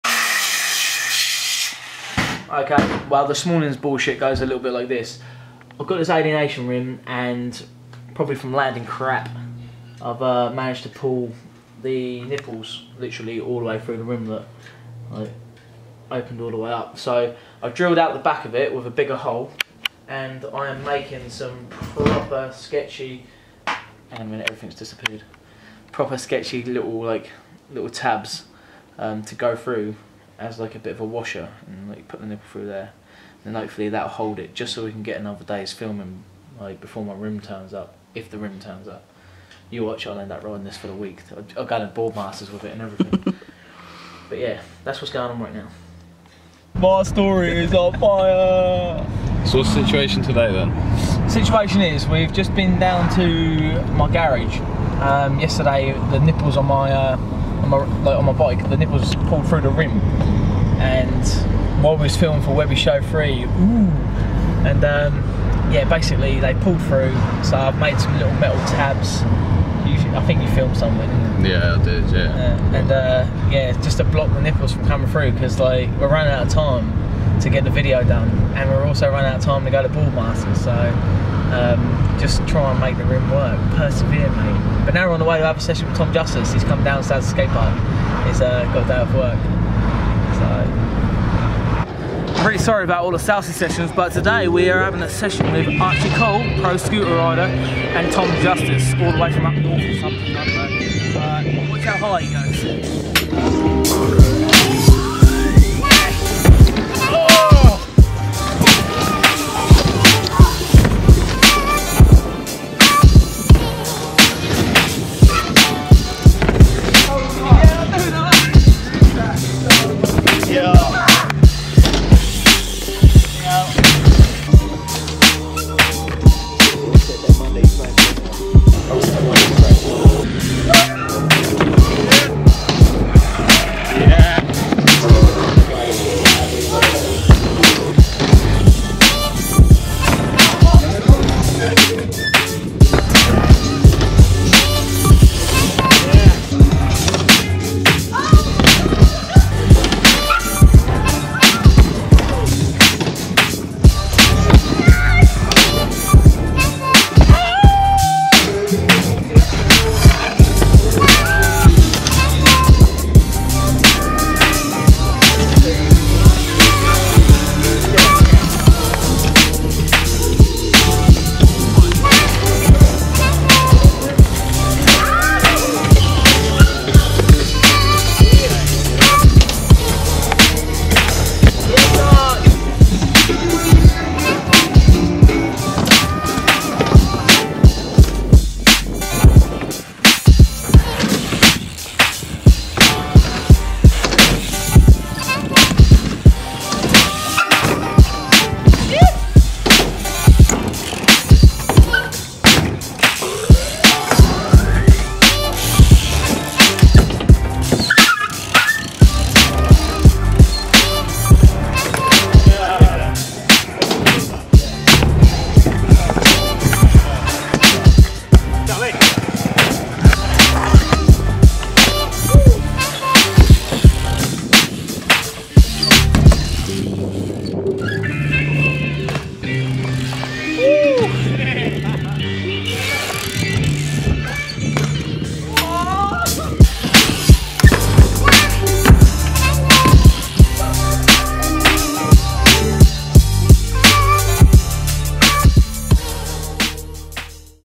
Mark Webb : Session With Tom Justice And Archie Cole
Session at Southsea skate park with Mark Webb, Archie Cole and Tom Justice.

For some reason reason when the video was posted it was fine. It got flagged up for copyright, since then the video has been glitching out. Sorry for the inconvenience.